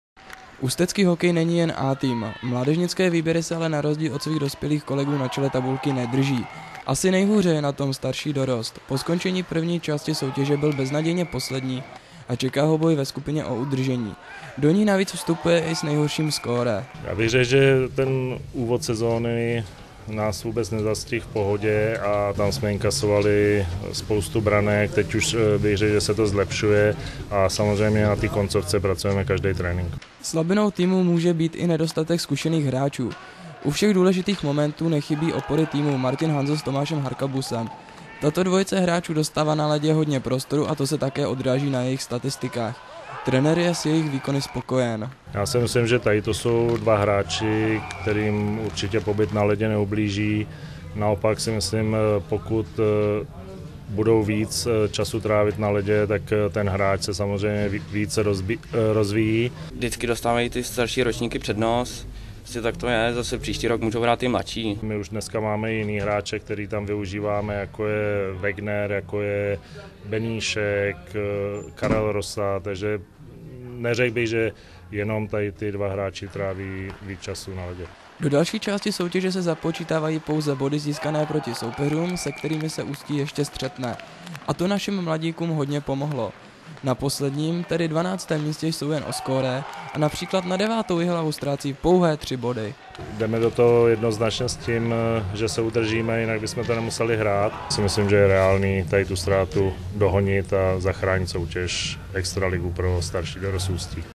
Starší dorost vstupuje do skupiny o udržení
krátké zhodnocení působení ústeckého staršího dorostu v extralize